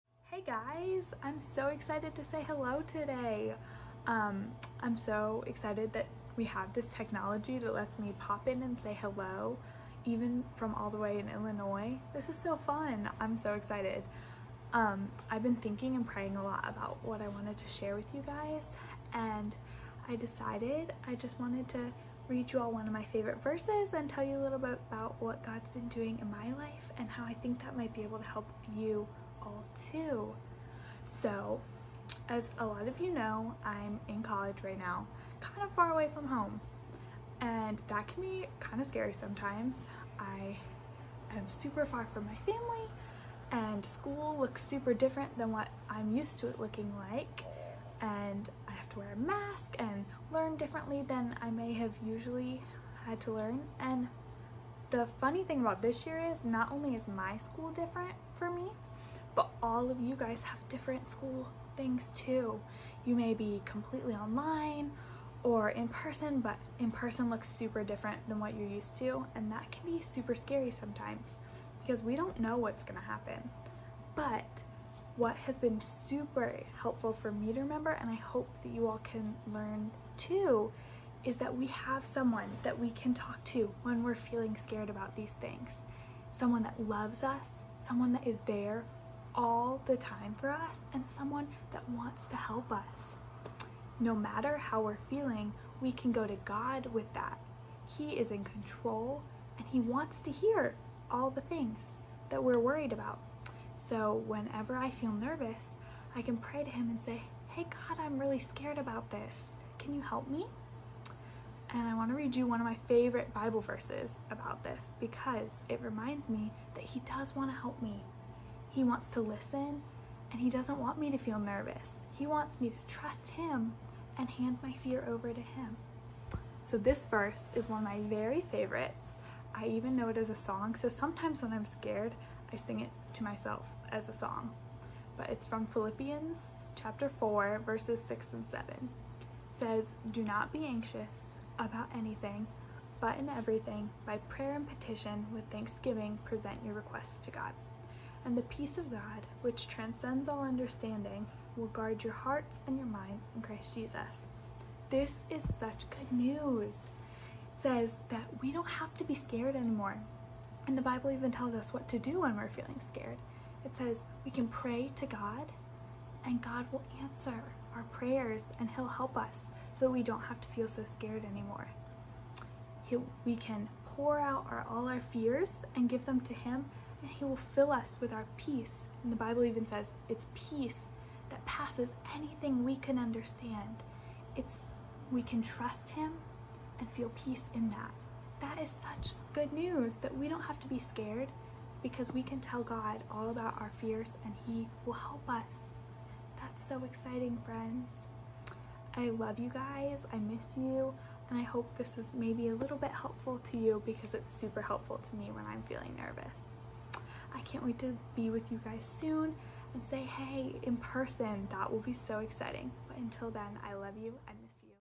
Children's Moment October 18, 2020
In a guest appearance in the Children's Moment Lola Sneddon gives a special word for our children from Wheaton College just outside of Chicago, Illinois.